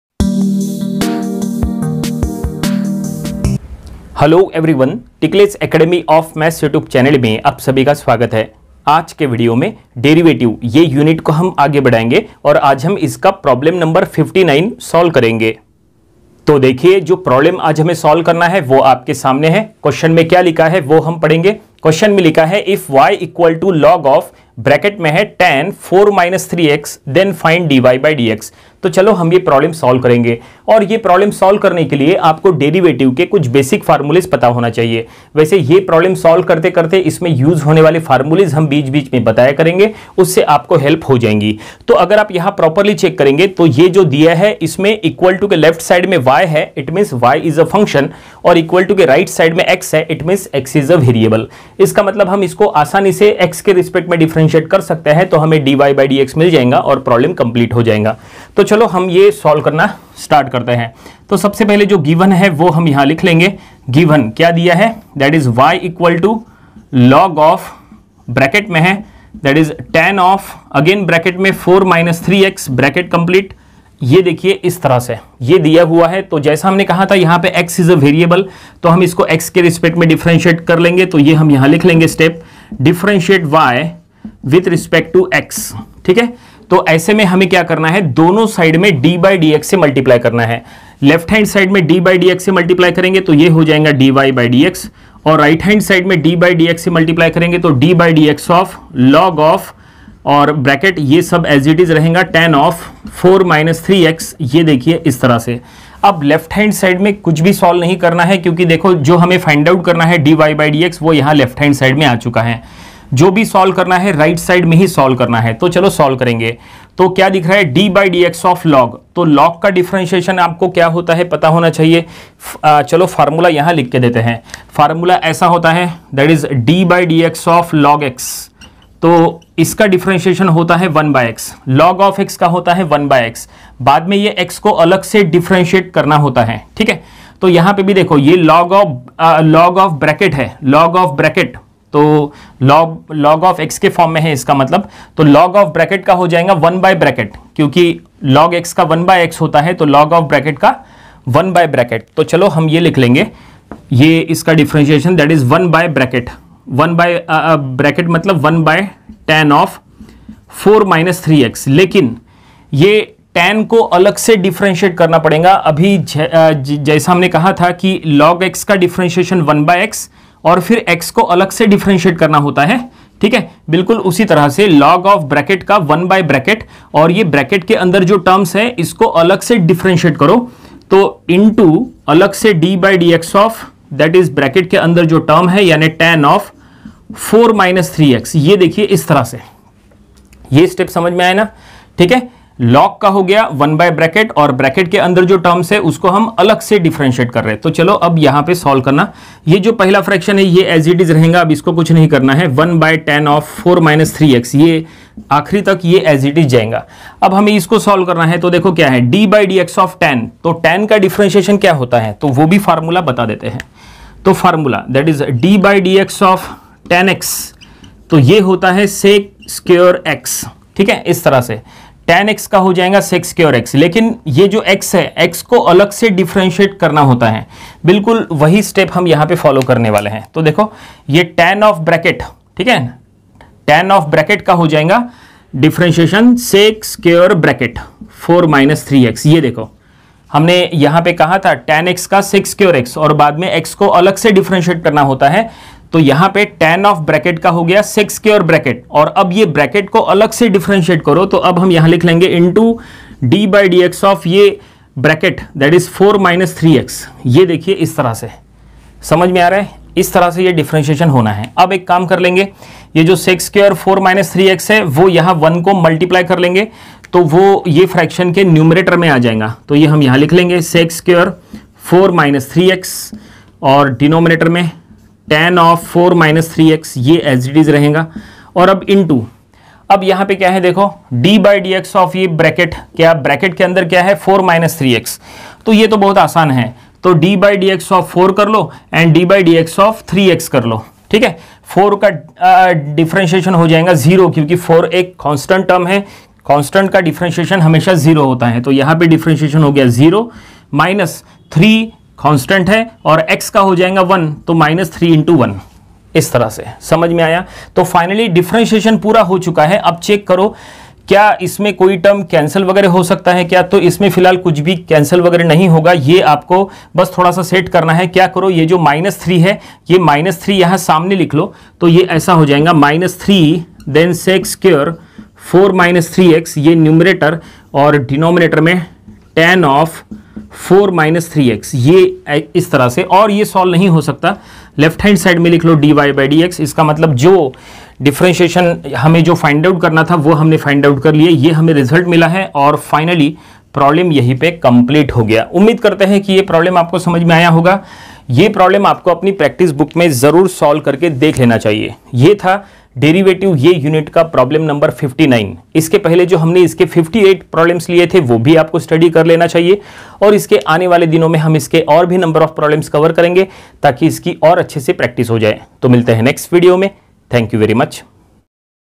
DERIVATIVE LECTURE 53 IN HINDI DIFFERENTIATION SOLVED PROBLEM 59
TODAY WE WILL STUDY 59TH PROBLEM ON DERIVATIVE.

PLEASE WATCH THE COMPLETE VIDEO TO CLEAR ALL YOUR DOUBTS.

FOR MORE OTHER TOPICS,
PLEASE VISIT THE PLAYLIST-SECTION ON MY CHANNEL.